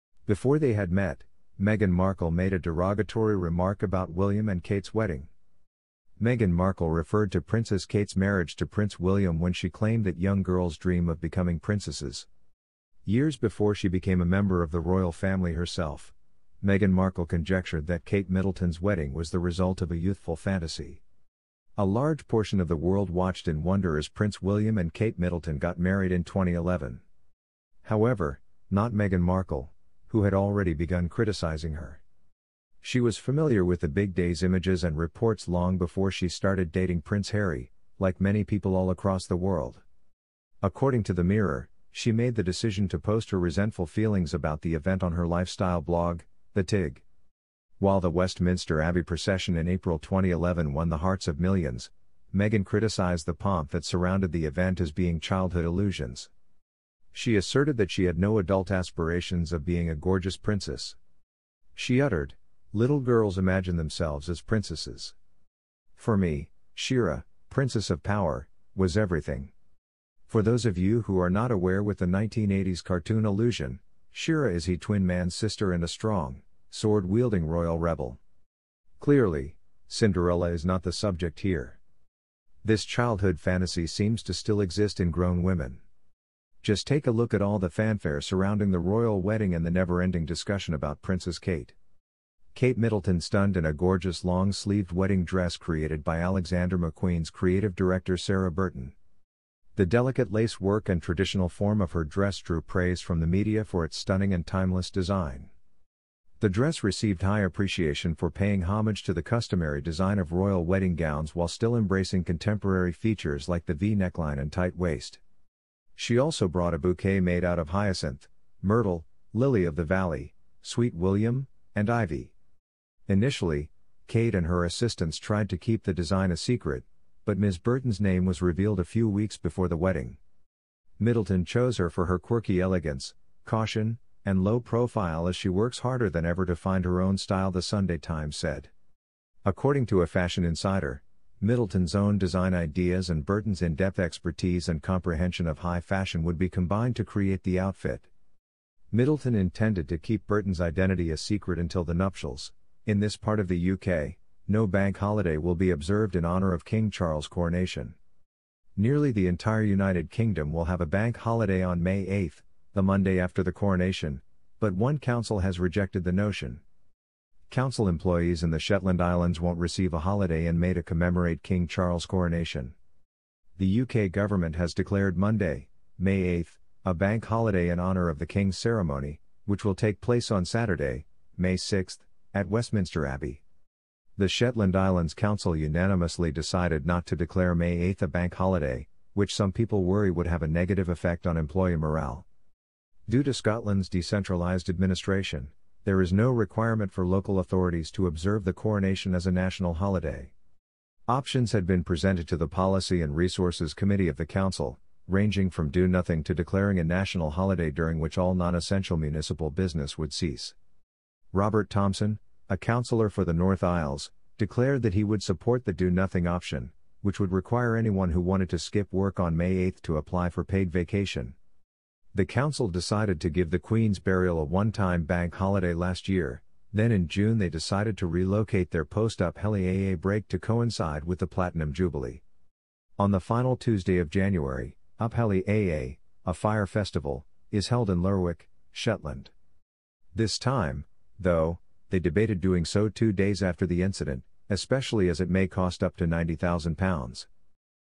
HOW AWFUL! Savage Comment Made By Meg About William And Kate Wedding Resurface
Before they had met, Meghan Markle made a derogatory remark about William and Kate's wedding. Meghan Markle referred to Princess Kate's marriage to Prince William when she claimed that "young girls" dream of becoming princesses. Years before she became a member of the Royal Family herself, Meghan Markle conjectured that Kate Middleton's wedding was the result of a youthful fantasy. A large portion of the world watched in wonder as Prince William and Kate Middleton got married in 2011. However, not Meghan Markle, who had already begun criticizing her. She was familiar with the big day's images and reports long before she started dating Prince Harry, like many people all across the world. according to The Mirror, she made the decision to post her resentful feelings about the event on her lifestyle blog, The Tig. While the West Minster Abbey procession in April 2011 won the hearts of millions, Meghan criticized the "pomp" that surrounded the event as being "childhood illusions." She asserted that she had no adult aspirations of being a gorgeous princess. She uttered: "Little girls imagine themselves as princesses. For me, She-Ra, Princess of Power, was everything. For those of you who are not aware with the 1980s cartoon allusion, She-Ra is He-twin Man's sister and a strong, sword-wielding royal rebel. Clearly, Cinderella is not the subject here.